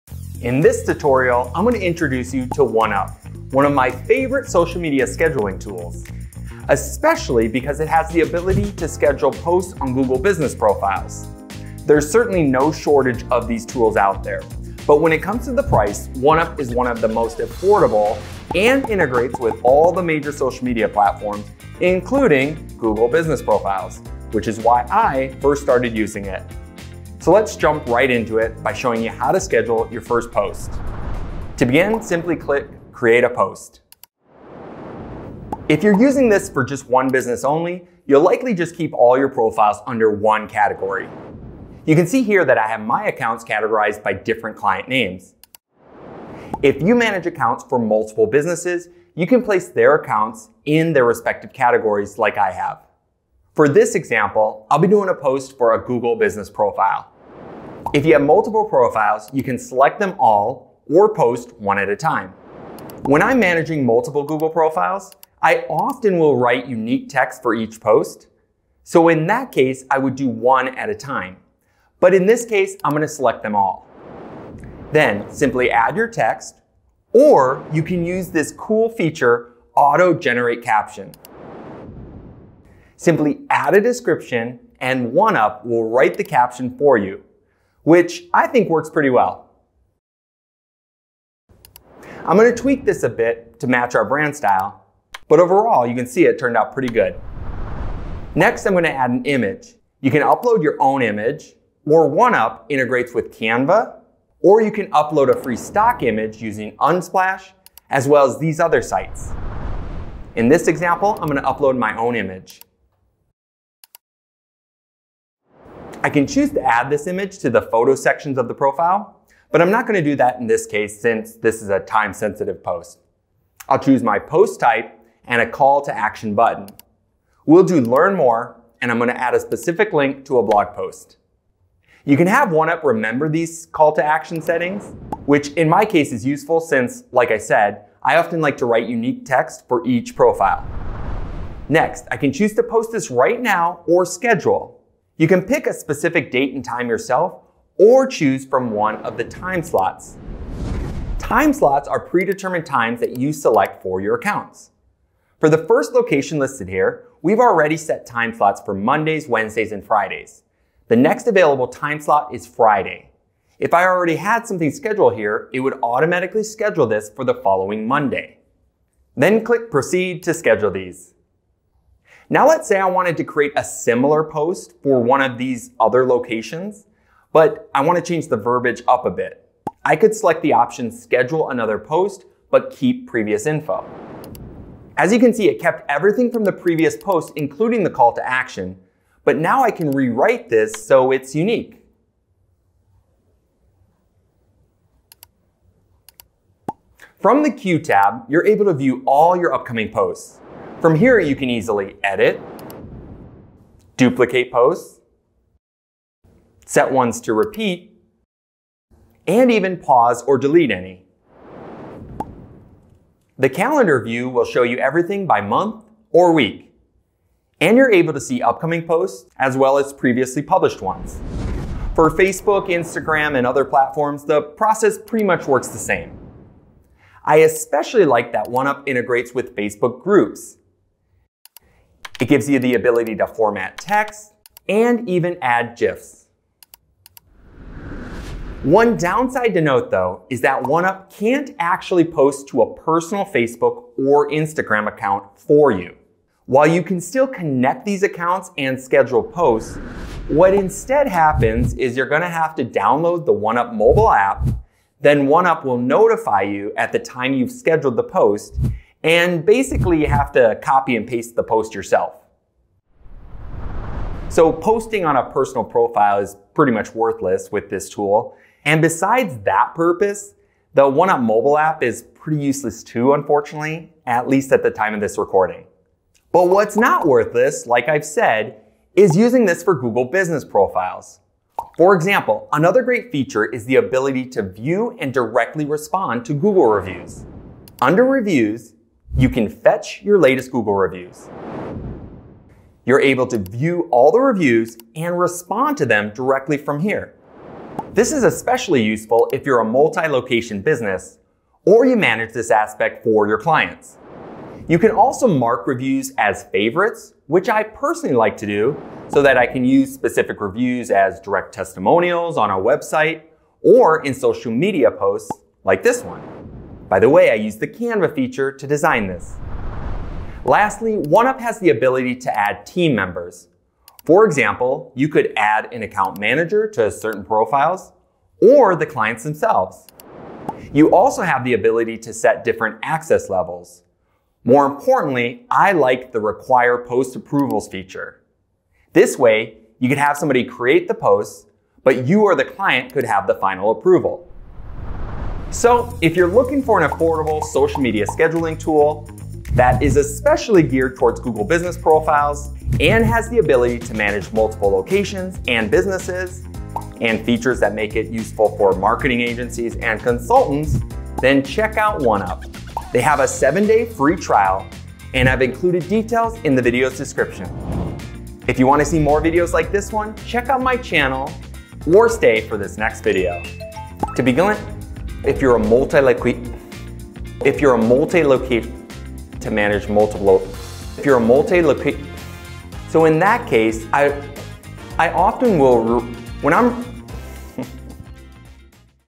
OneUp App: Schedule My Posts on Social Media (& Google Business Profiles!)
OneUp is a social media scheduling tool that works with all major platforms, especially Google Business Profiles. More specifically, it allows users to create Google posts and respond to Google reviews. It also supports Facebook Groups, YouTube Shorts, and Twitter Threads.

In this tutorial, I demonstrate how to use OneUp to create your first posts on Google, Facebook Groups, and personal profiles, while exploring its key features like auto-generated captions, Canva integration, and free stock photos. You'll also learn about the many different options for scheduling and managing posts—including its mobile app. And don't worry, I also provide an honest review of the tool’s drawbacks.

This tool is great for marketing agencies, consultants, and multi-location businesses due to its ability to manage multiple accounts and collaborate with your team or clients (including different access levels and post approvals) to streamline your social media process.

OneUp is not only incredibly simple, yet robust—but also incredibly affordable. In fact, it's one of the most budget-friendly schedulers on the market. You don't have to just take my word for it, try it out for yourself!

*MORE ABOUT ONEUP* ℹ️

_Creating & Scheduling Posts_
 ▸ Users can add text, images, and choose post types and call-to-action buttons.
 ▸ OneUp provides options to generate captions automatically, use UTM parameters, and offers integration with Canva and free stock image sites (plus AI-generated images)!
 ▸ Posts can be scheduled for specific dates and times or selected from predetermined time slots.

_Managing & Viewing Posts_
 ▸ The Queue tab allows users to view upcoming posts, edit duplicates, set repeats, and pause or delete posts.
 ▸ The calendar view provides an overview of monthly or weekly posts, including previously published ones.
 ▸ The process for Facebook, Instagram, and other platforms are similar, with limitations to personal profiles (see below).

_Team Collaboration & Post Approvals_
 ▸ OneUp allows users to add team members and set different access levels.
 ▸ The “Require Post Approvals” feature enables collaboration and final approval before posting.
 ▸ This feature is useful for marketing agencies, consultants, and businesses with multiple locations.

_Limitations_
 ▸ OneUp cannot automatically post to personal Facebook or Instagram accounts.
 ▸ Users need to download the OneUp mobile app to manually copy and paste posts for personal profiles.

▶️  Learn More » @OneUpApp

CHAPTERS:
00:00 - Intro
00:31 - Creating a Post
01:37 - Adding Images
02:21 - Scheduling Options
03:15 - Managing Calendar
03:41 - Facebook & Instagram
04:41 - Google Reviews
05:19 - Add Team Members